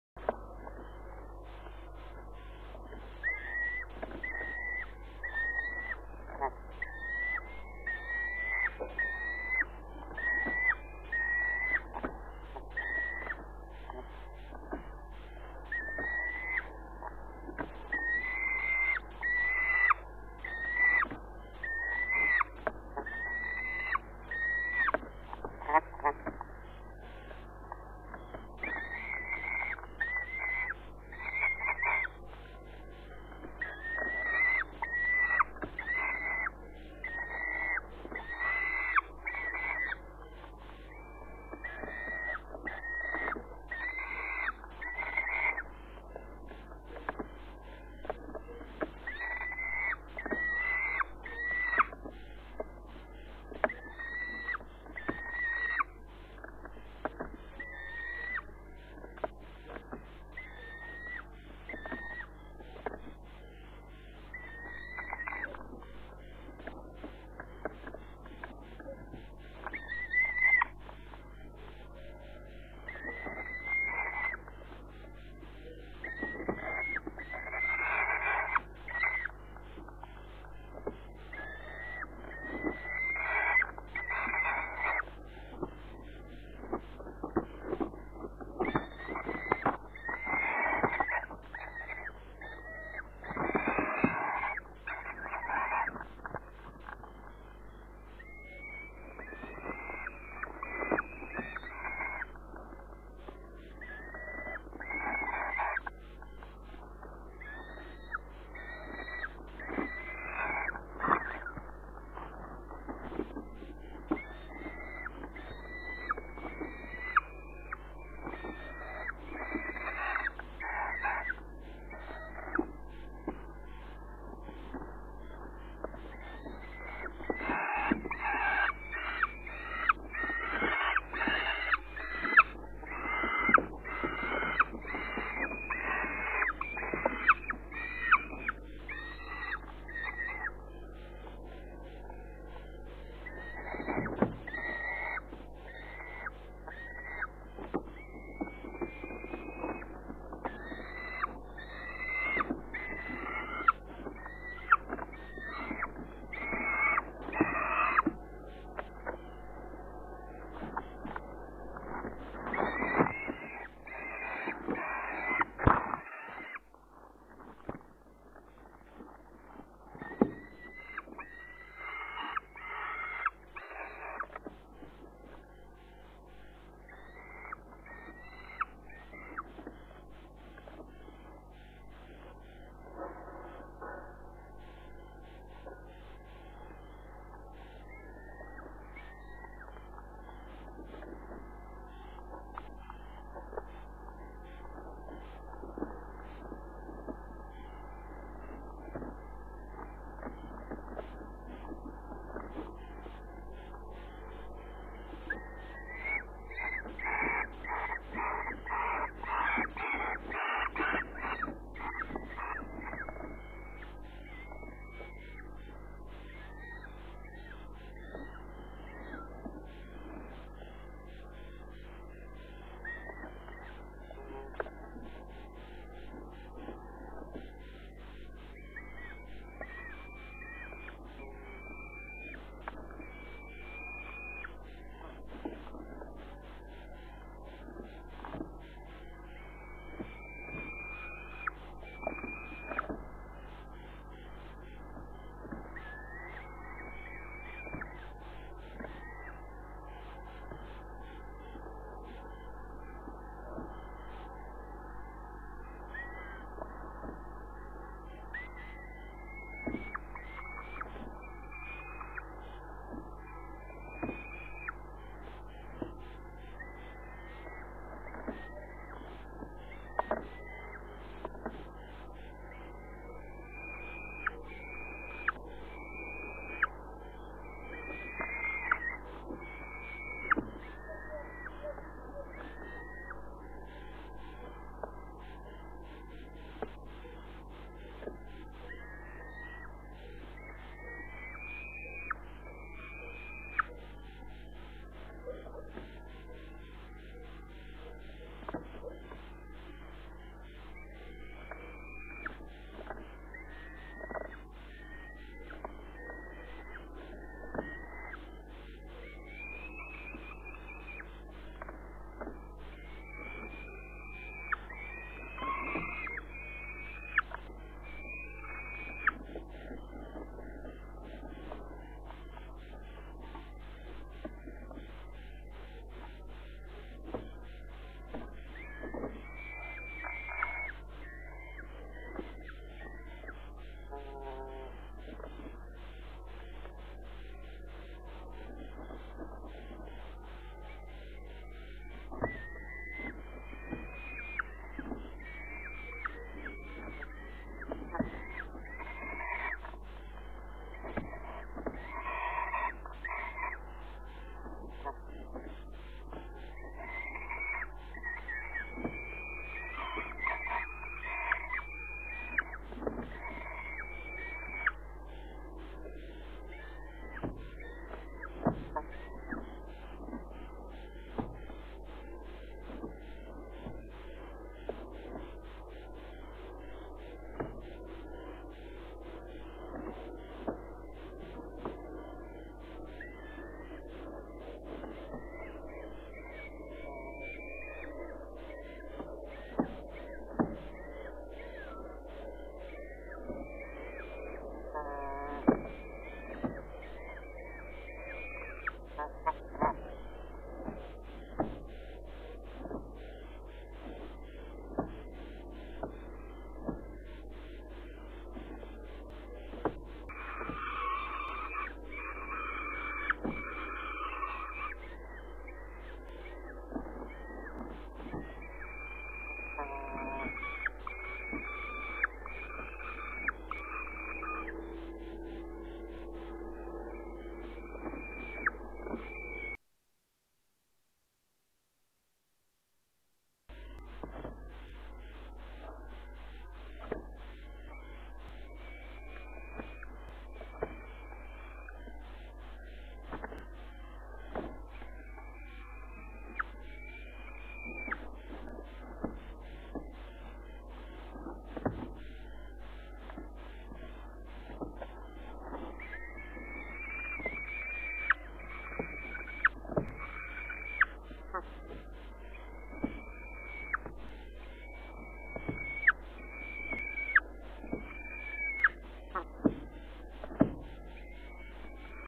130616- Pa voert restprooi, eet zelf ook flink
vooral 2R krijgt hapjes